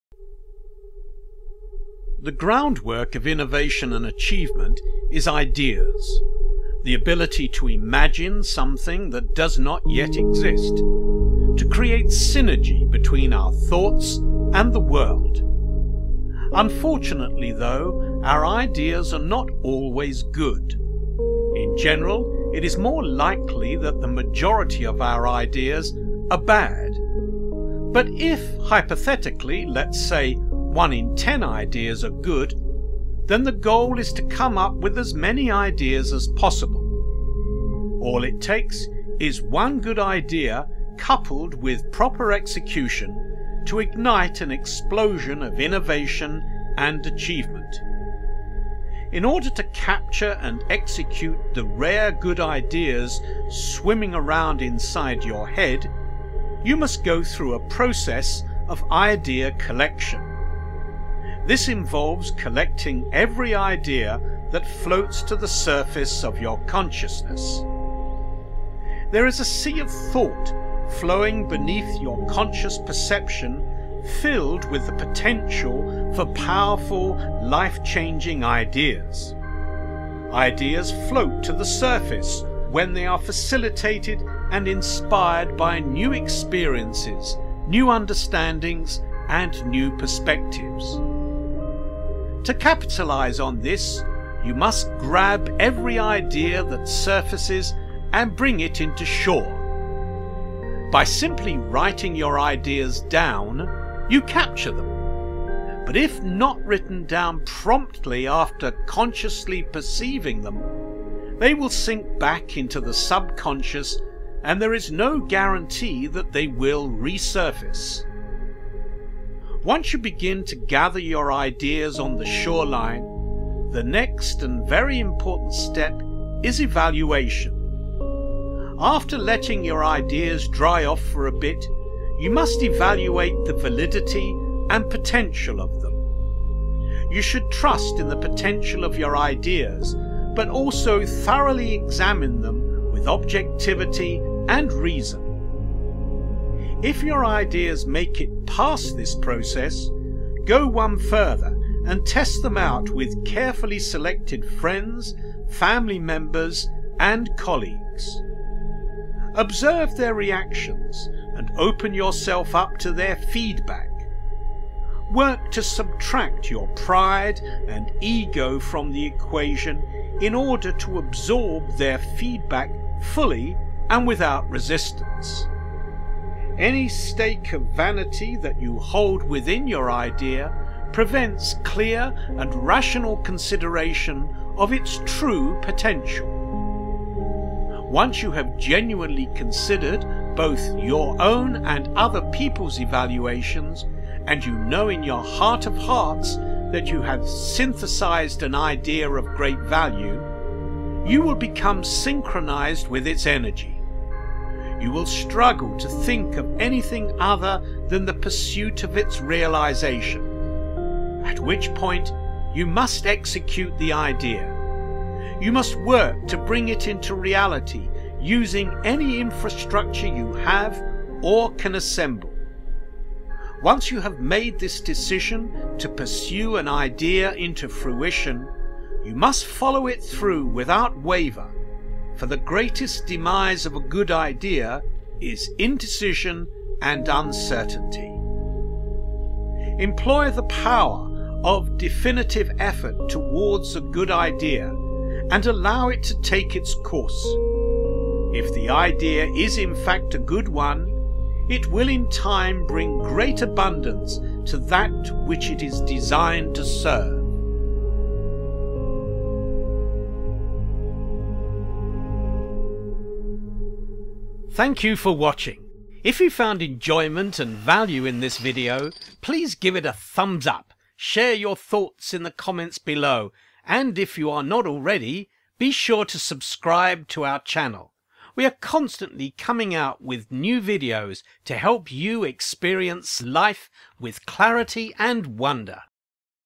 How To Come Up With Good Ideas - The Art Of Critical Thinking
Our brains are idea machines. They are constantly perceiving and interpreting the world around us, creating building blocks for strong, successful, and innovative ideas. It is our duty to our self and the rest of humanity to pull from those ideas, use our ability to think critically, and bring them to the world.